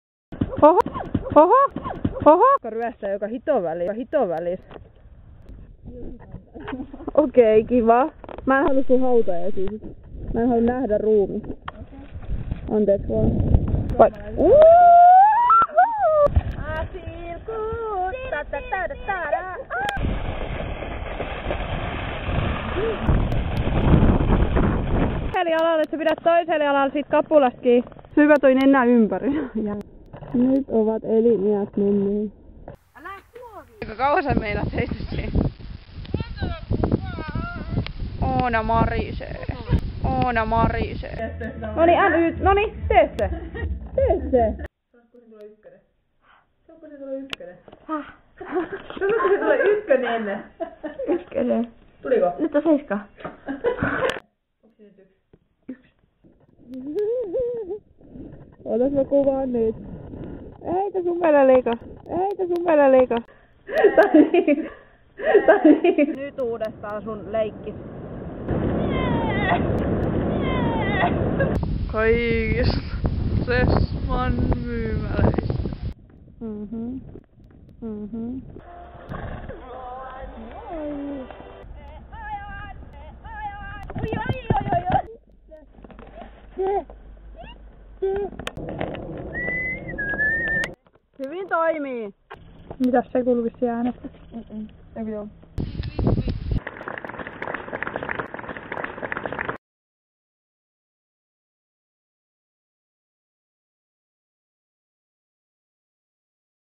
Säätöä vain!
Joops, eli päätin tehdä KERRANKIN jonkin erilaisen videon. No päädyin sitten kokoamaan joitakin ääniä, äänähdyksiä videolle ja tällainen siitä sitten tuli (:
Olisi ollut vielä paaaljon lisää materiaalia, mutta alkoi ohjelma herjata joten päätin lopettaa enne kuin menee koko video pilalle. Hope u like it!

Kiitos videoissa mukana olleille kavereille: mm. Päiville ja Oonalle jotka minun lisäksi eniten ovat videossa mukana. Lisäksi myös Salli (ääni ainakin kerran) ja siskoni yhdessä kohtaa :)